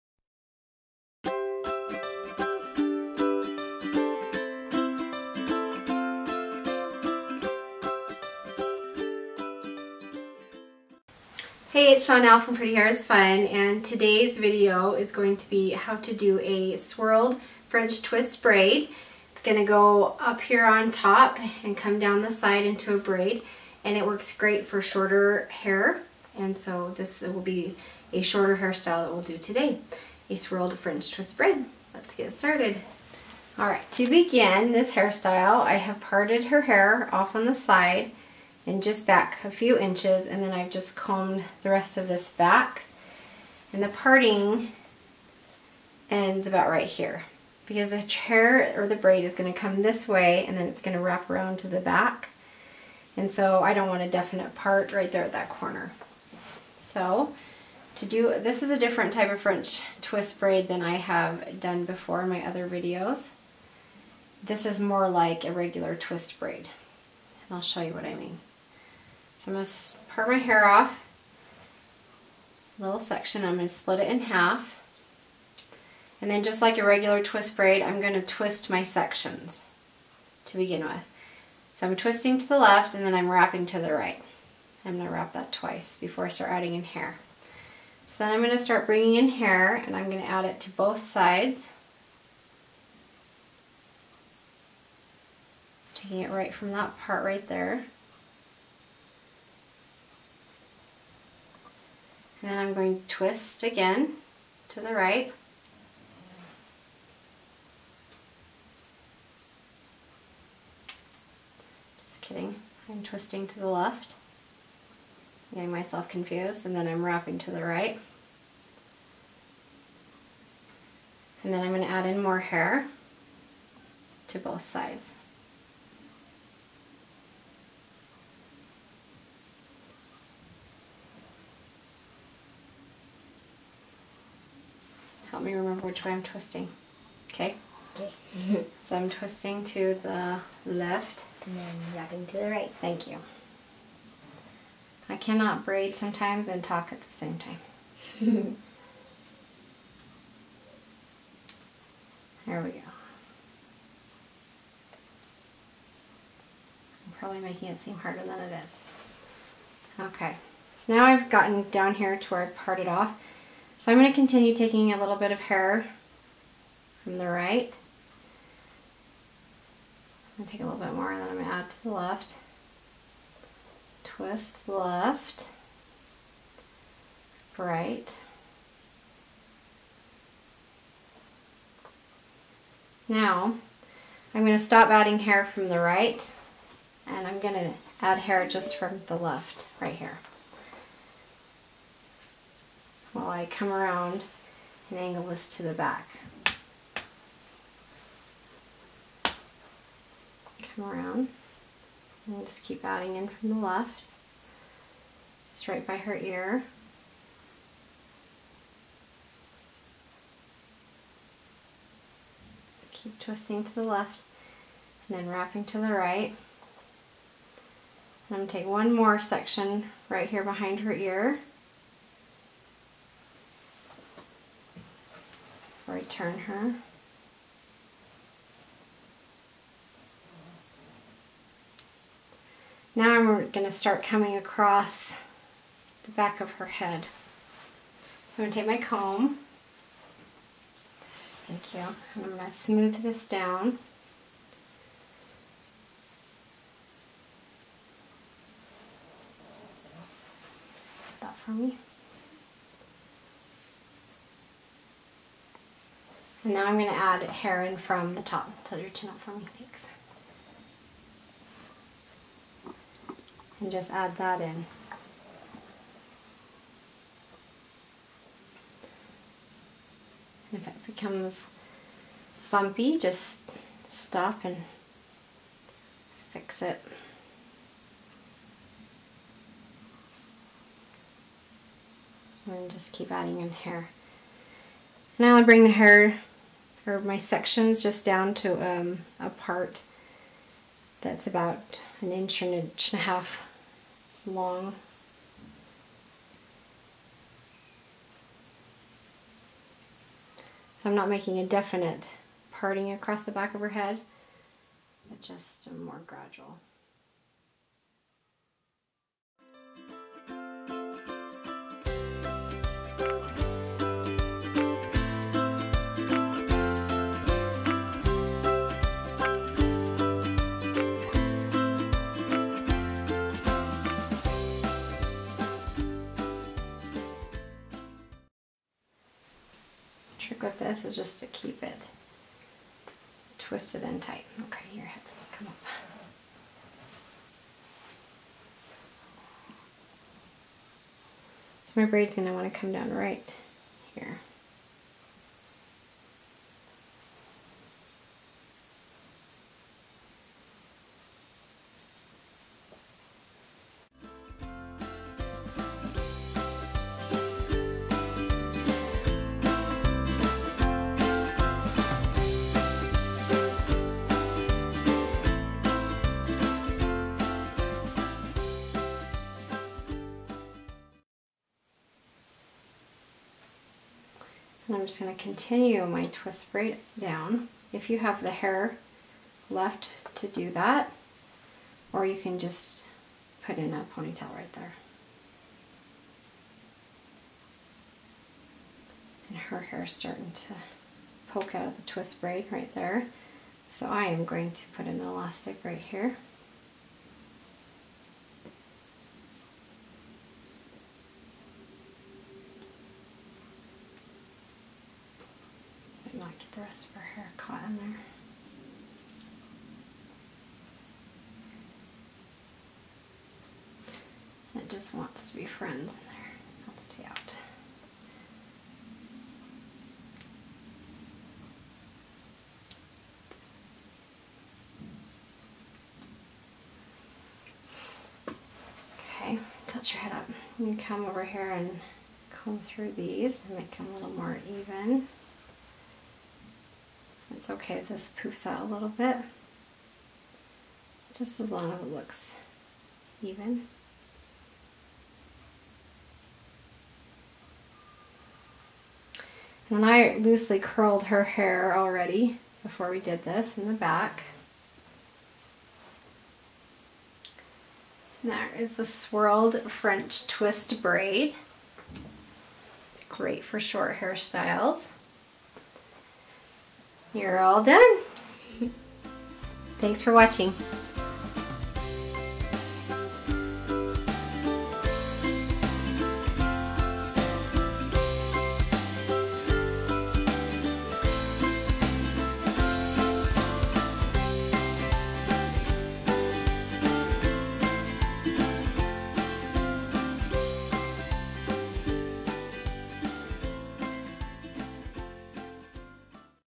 How To: Swirled French Rope Twist Braid | Pretty Hair is Fun
Learn how to do this cute swirled french rope twist braid. It wraps around the top of the head and ends on the side. This braid is great for shorter hair lengths, and can be great for school mornings, a wedding, or a bridesmaid hairstyle, whatever you like! You'll need one elastic and about 10 minutes to do this braid. Enjoy!